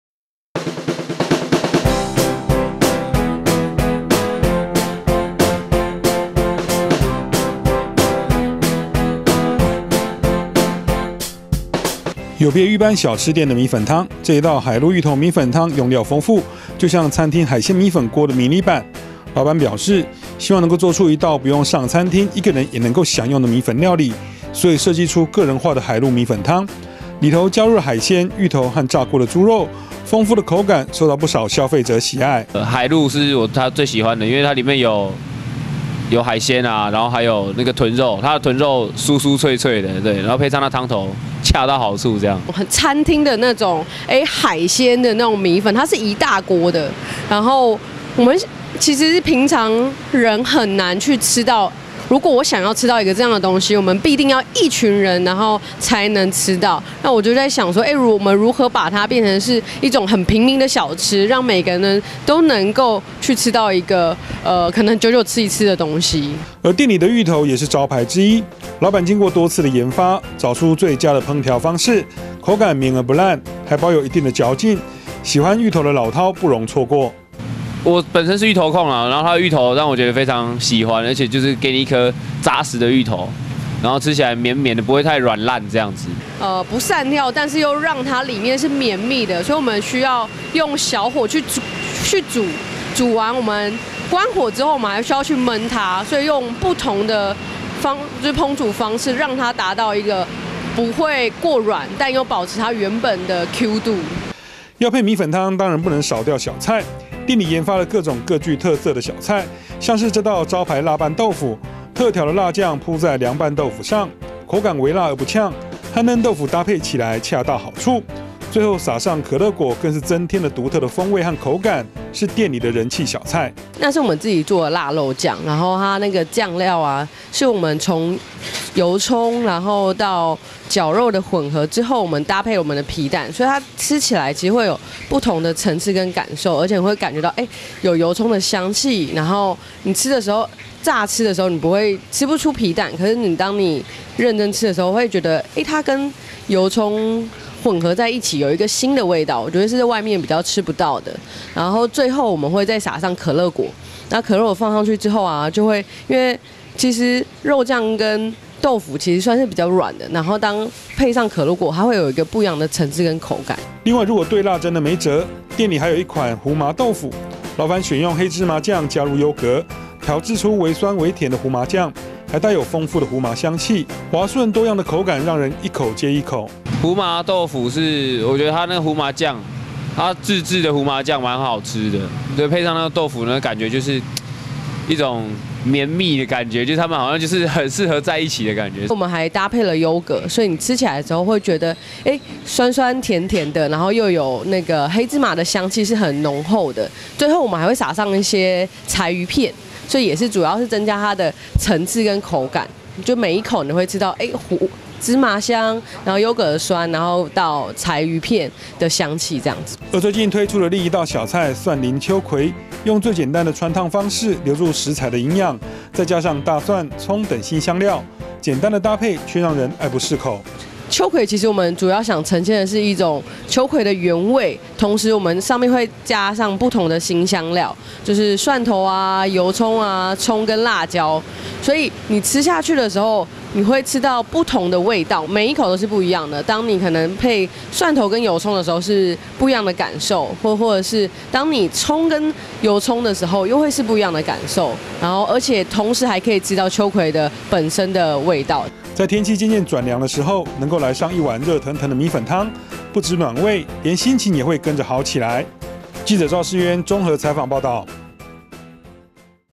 捷運 好吃美食2020|海鮮米粉新吃法 一個人也能獨享美味
翻轉傳統小吃米粉湯 創意小菜吸客來
(記者 趙世淵 中和報導) 米粉湯是很常見的台灣小吃，在中和景安站附近卻有家米粉湯店，將海鮮、豬肉、芋頭等食材加入其中，頓時升級成在餐廳才能吃到的綜合米粉湯，再搭配自行研發的涼拌豆腐、秋葵等小菜，成為相當有特色的小吃店。